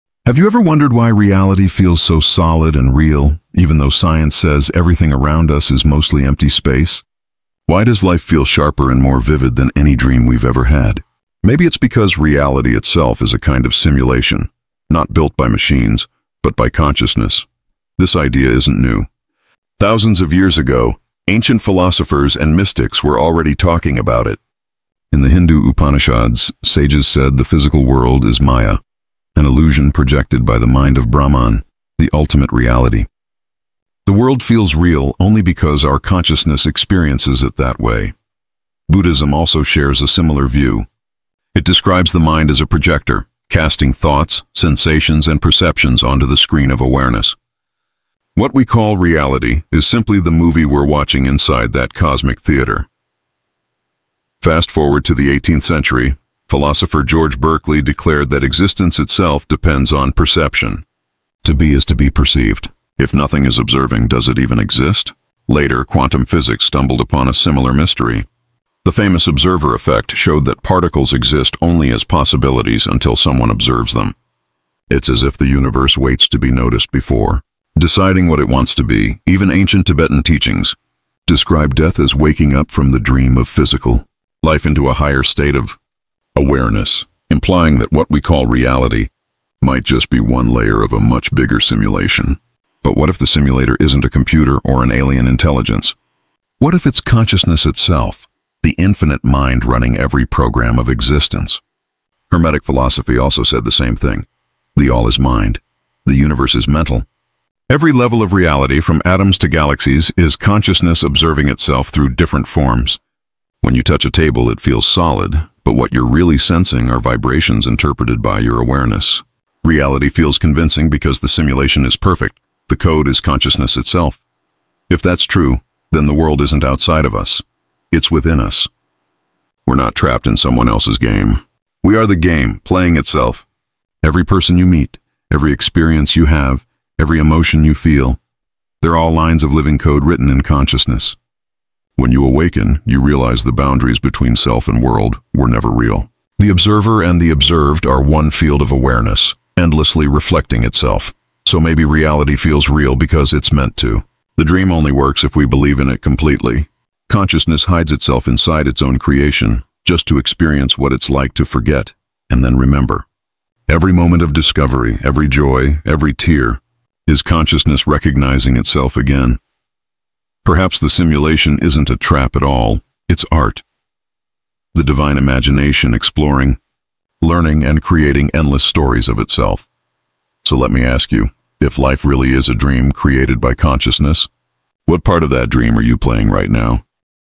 The Paradox Of Reality Explained in 3 Minutes
Reality feels solid, but what if it’s not what it seems? This video dives into the paradox of existence — exploring how consciousness, perception, and illusion shape the world we call “real.” From ancient mysticism to modern physics, discover why reality might be the greatest illusion ever created.